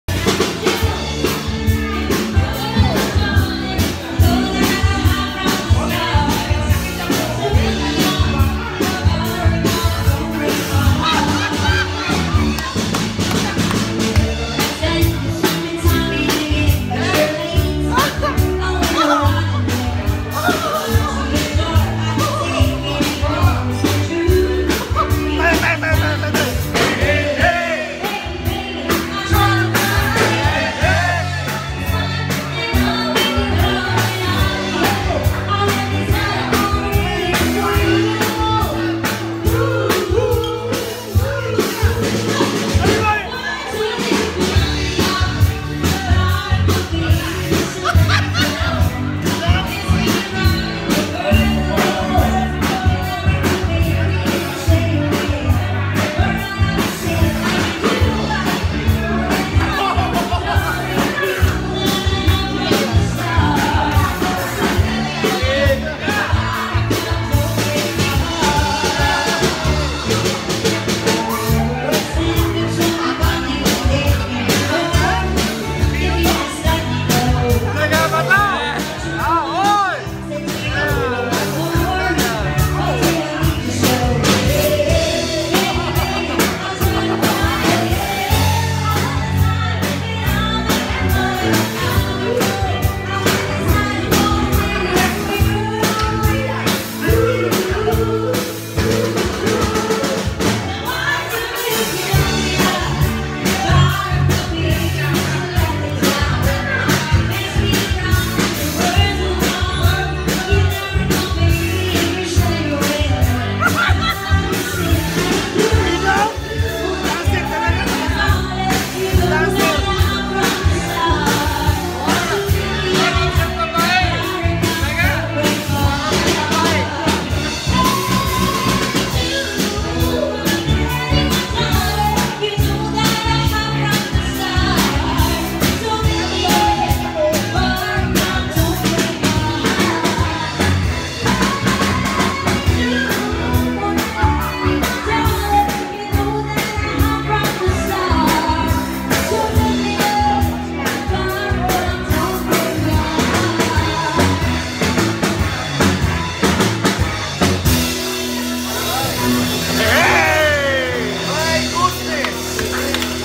VAC. IN PHILIPPINES MARCH-APRIL 2017.
Sayang naman itong video n ito kung nka store lang sa memory card. bakasyun taong 2017 ng March to April. sana mapanuod nyo ito lalo na ito mga nsa video n ito.